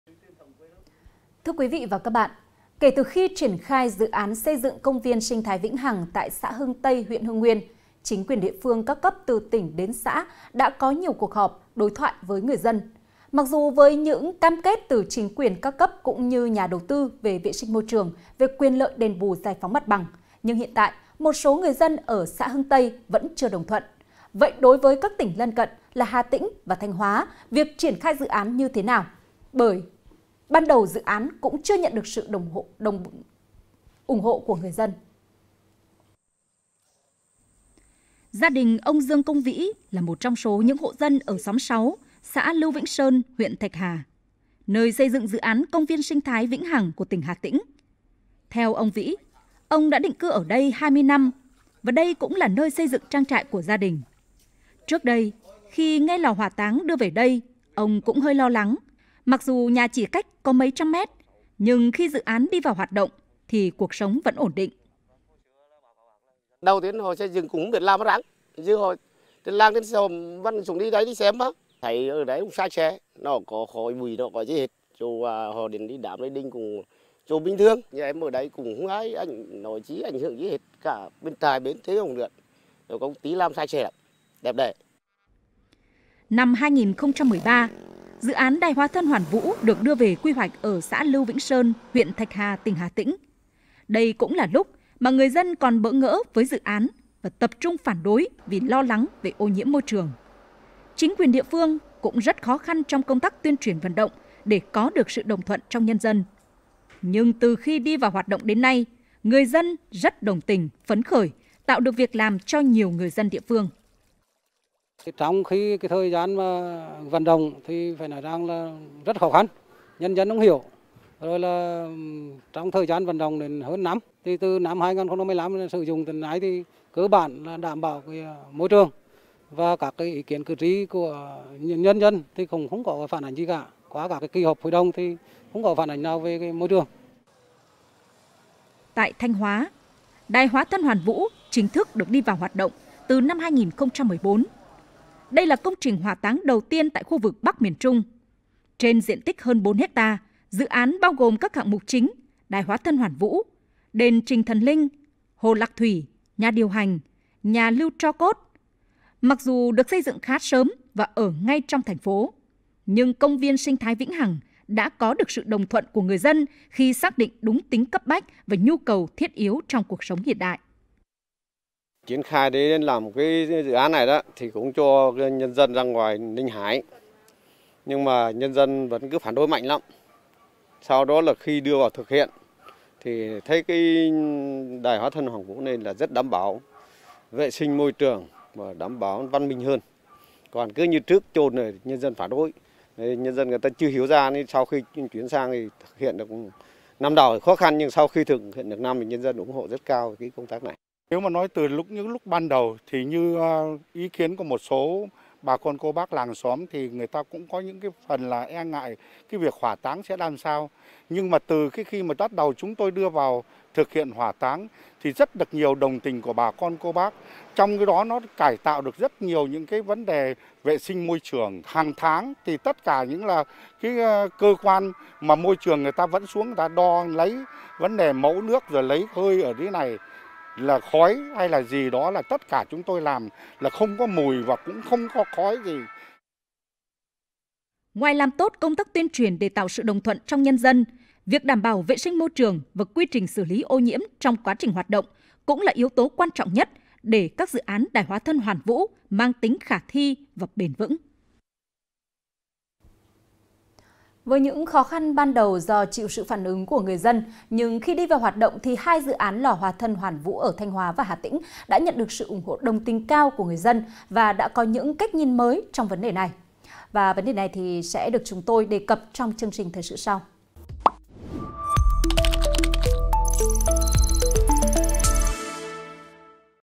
Xây dựng công viên sinh thái vĩnh hằng nhìn từ các tỉnh Thanh Hóa, Hà Tĩnh
Kể từ khi triển khai dự án xây dựng công viên sinh thái vĩnh hằng tại xã Hưng Tây, huyện Hưng Nguyên, chính quyền địa phương các cấp từ tỉnh đến xã đã có nhiều cuộc họp, đối thoại với người dân. Mặc dù với những cam kết từ chính quyền các cấp cũng như nhà đầu tư về vệ sinh môi trường, về quyền lợi đền bù giải phóng mặt bằng… nhưng hiện tại, một số người dân ở xã Hưng Tây vẫn chưa đồng thuận. Vậy đối với các tỉnh lân cận là Hà Tĩnh, Thanh Hóa, việc triển khai dự án như thế nào? Bởi ban đầu dự án cũng chưa nhận được sự ủng hộ của người dân.